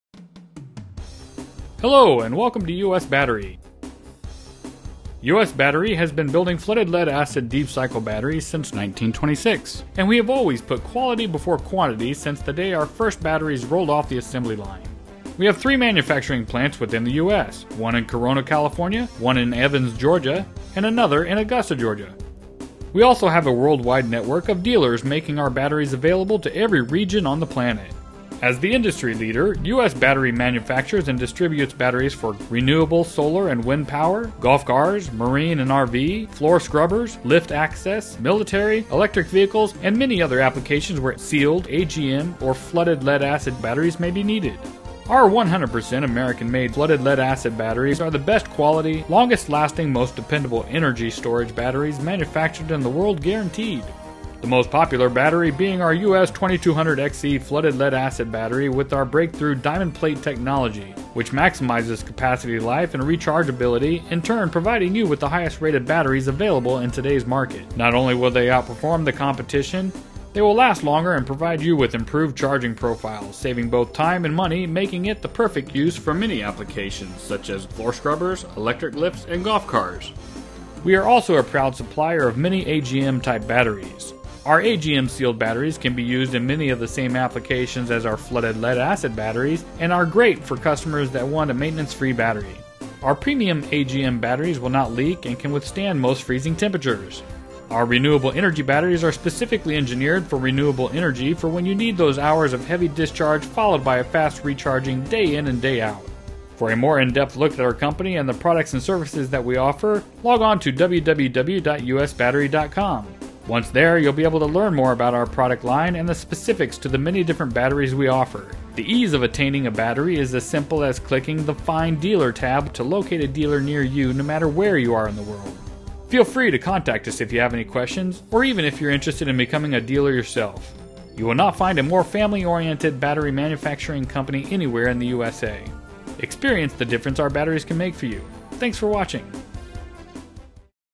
U.S. Battery introduction - US Battery is the World Leader in Deep Cycle American made batteries
This video is an introduction to U.S. Battery, the world leader in deep cycle battery manufacturing.